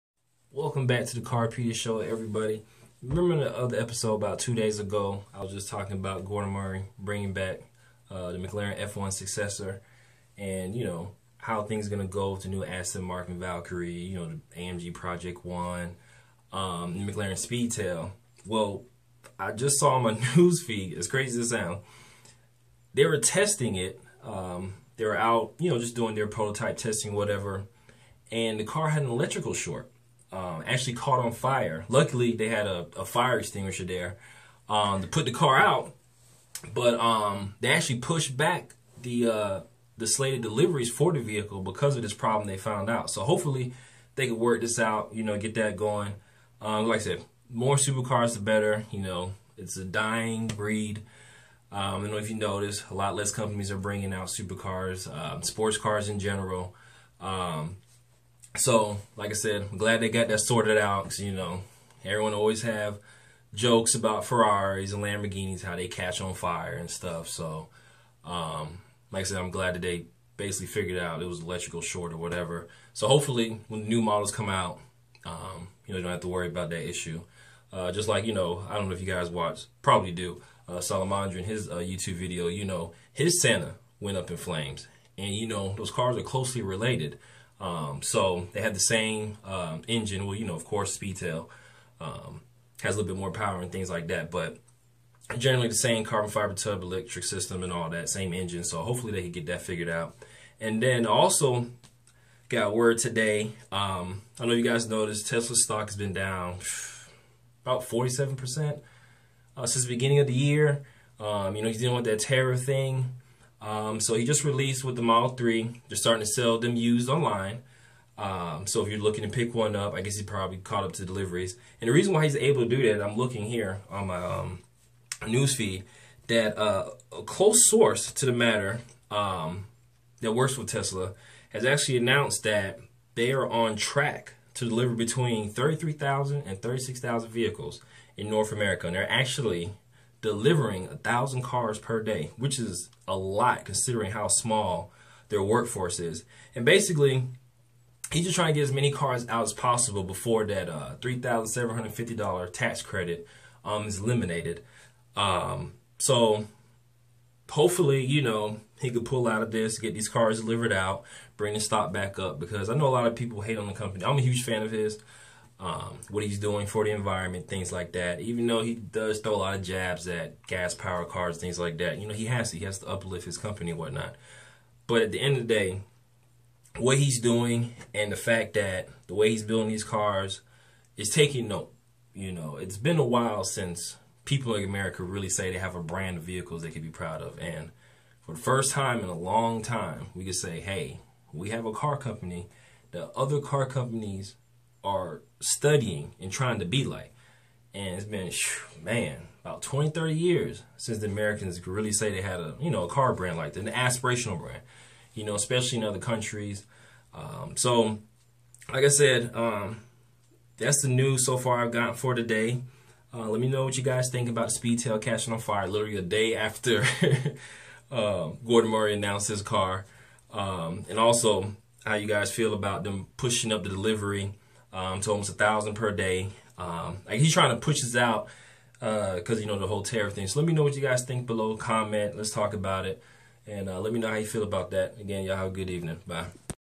McLaren Speedtail Catches On Fire And Tesla Deliveries Up To 1000 Cars Per Day
McLaren Speedtail prototype catches on fire during testing while parked. The head of Mclaren Global and North American communications stated it was just from an electrical fault but since the car was parked at a refueling station in the UK they backed the car up and applied the fire extinguisher before things got out of hand.

Tesla is on track for a record quarter with deliveries after promising 33,000-36,000 cars this month. Sources at Tesla say they have been delivering 1000 cars per day so far this entire month to keep up with demand as well as to meet their June goal they set. Just yesterday they were denied the china tariff exemption they applied for on the Model 3 on certain components and the $3,750 tax credit from the government on zero emissions vehicles is about to expire for Tesla soon. Hopefully this news will help investors feel more at ease with Tesla stock and it begins to raise back up to where it was at before the slide began in earlier this year.